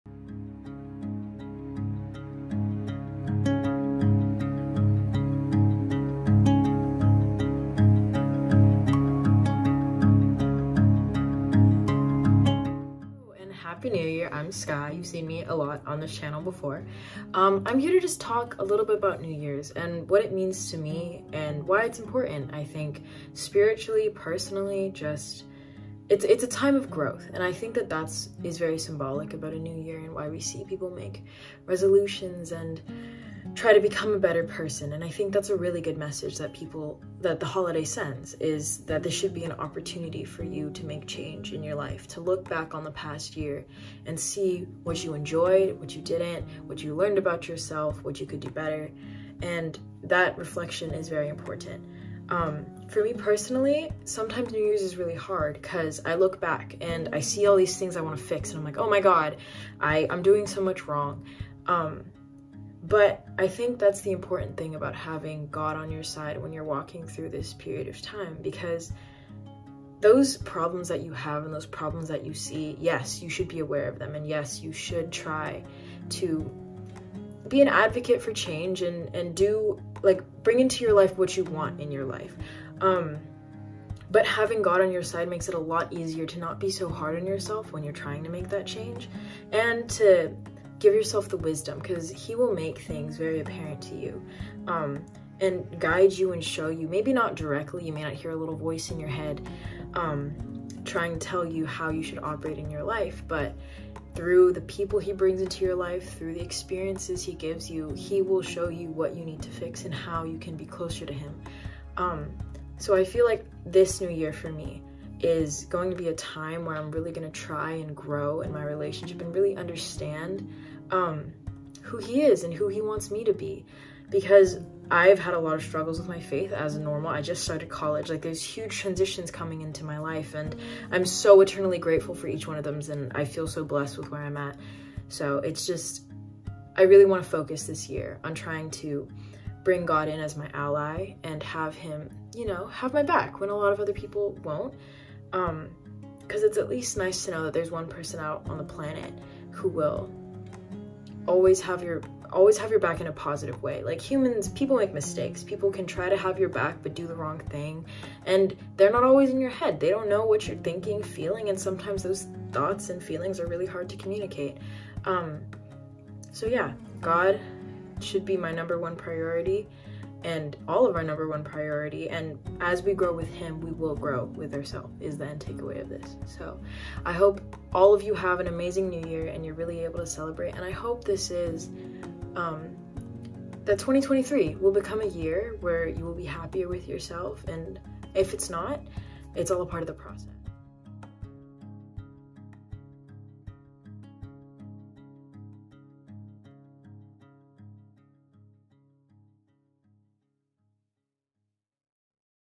Walking into this new year
2023 is almost here! Are you feeling excited, nervous, or just the same as always? In this video, Sky talks about the importance and hardship that the new year can bring. We hope all of you have a fantastic 2023. See you next year!

Follow our youth group:
Facebook: @Student Ministry at Oneonta